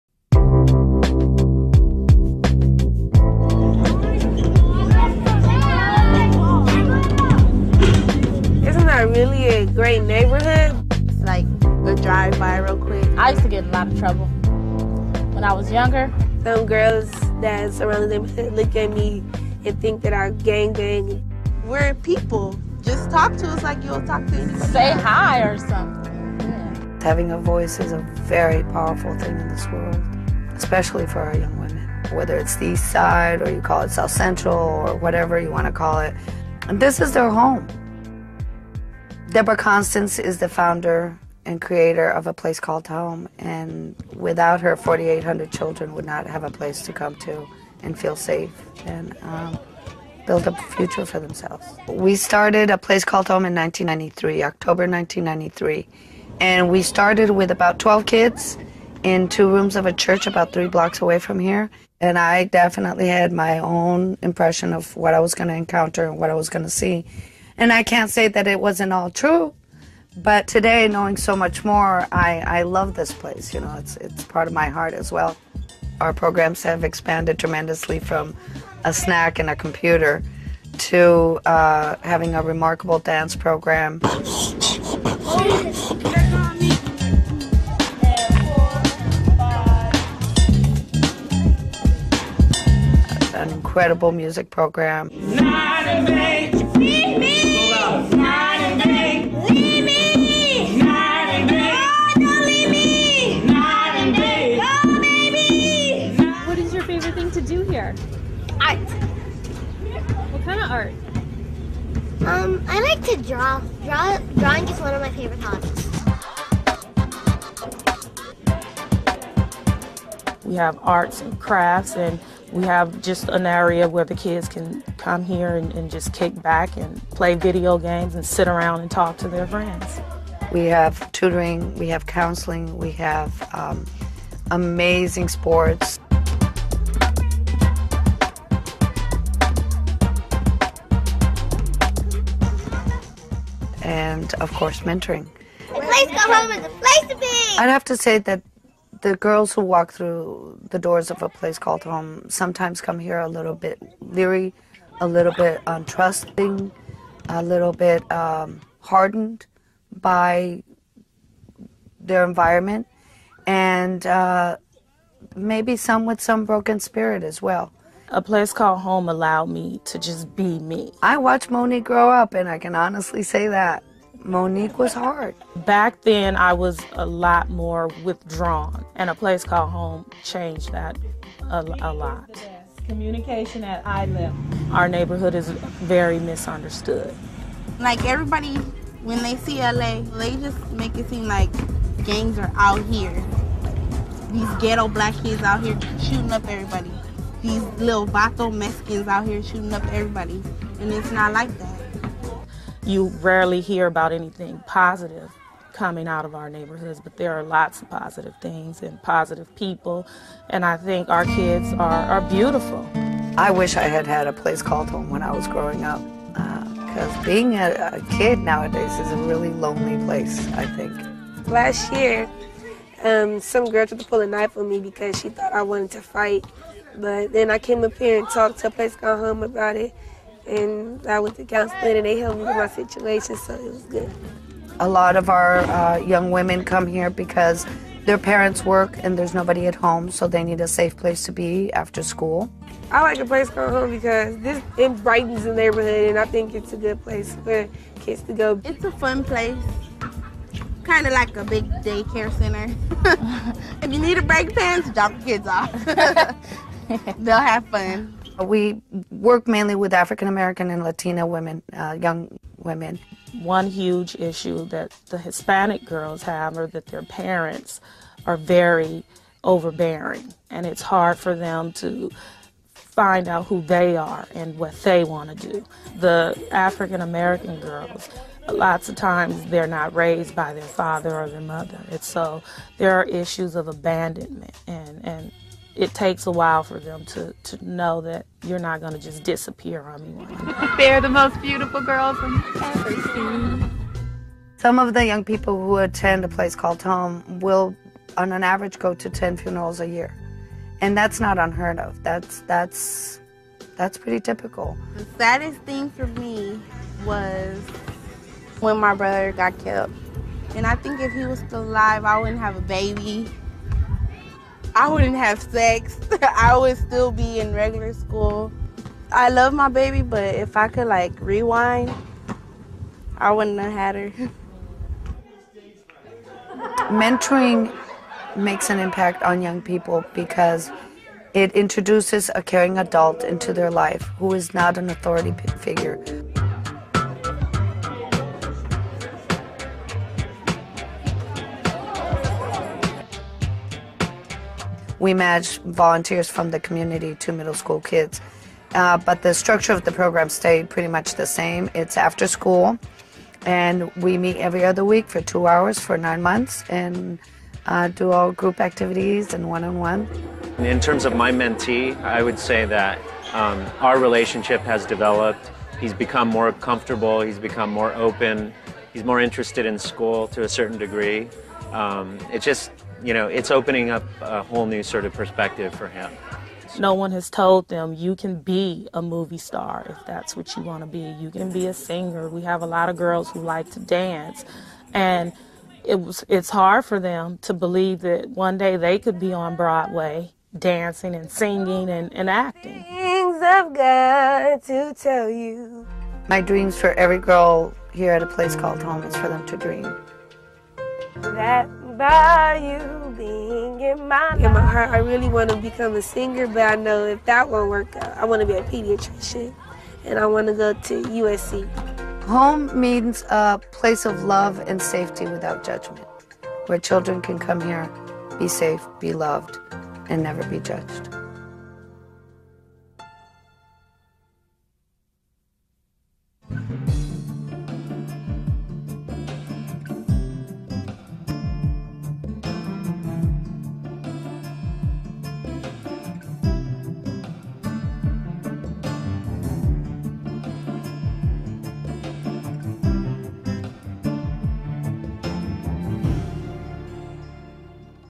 A Place Called Home
The vision of APCH is to revitalize the community of South Central Los Angeles in a continued effort to provide inner-city youth with a safe environment and a safe place to develop and grow. This vision includes growth and expansion of the APCH center, community park and garden. It is also part of the vision to rehabilitate a six-block radius around the center within the next five years. One block at a time, the community will be slowly cleaned up, changing perceptions and forming pride in the neighborhood. It is our hope to work with the community and collaborating agencies in an effort to make the inner-city a better place for the children and everyone.